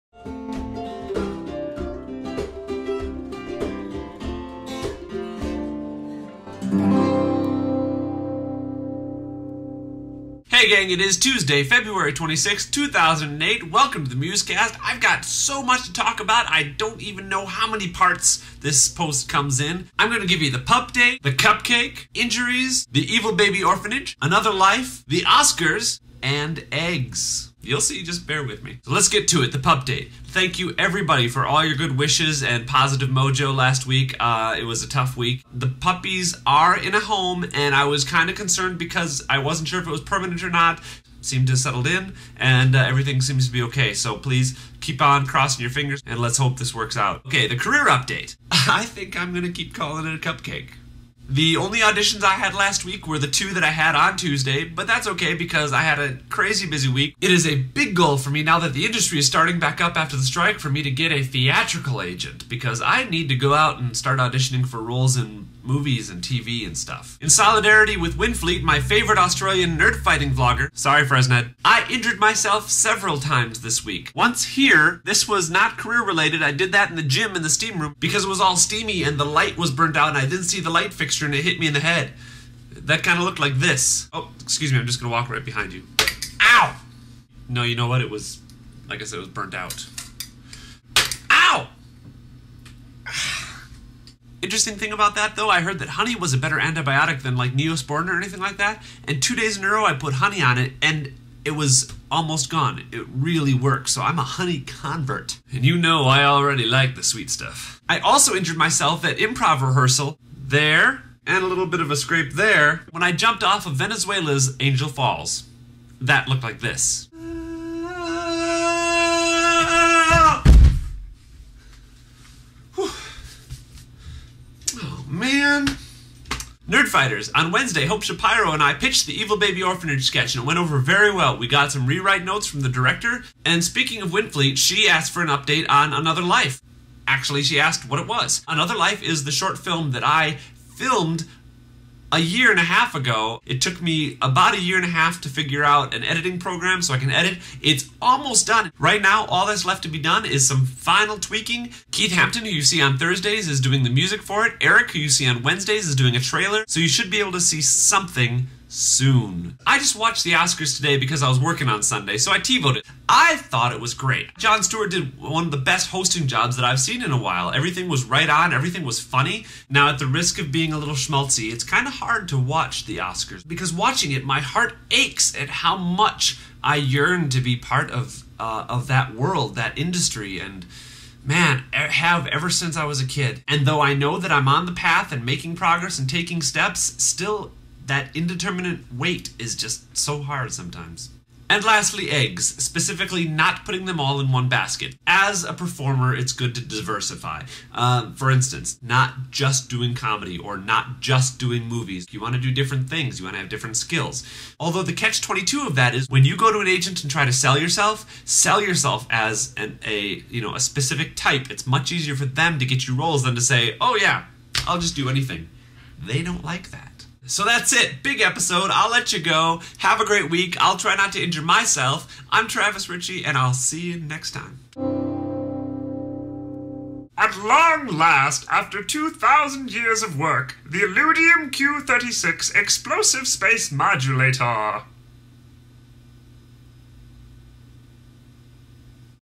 106 Musecast - February 26, 2008 ~ There are 7 parts!
Travis Tuesday!

Today's post might not make any sense... I think I'm actualyl coming down with a bit of a cold.  As usual I crammed a bunch of stuff into this episode and had to cut it down.  I was going to leave in somethign and just go over the 4 minute rule/guideline, but then I decided it was best not to.

Seven Parts!
1) The Pupdate
2) The Cupcake (career update)
3) Minor injuries - Honey is awesome!
4) Evil Baby Orphanage sketch update!
5) "Another Life" short film update!
6) The Oscars - Schmultz alert!
7) Lesson: be a diverse performer, but promote yourself specifically.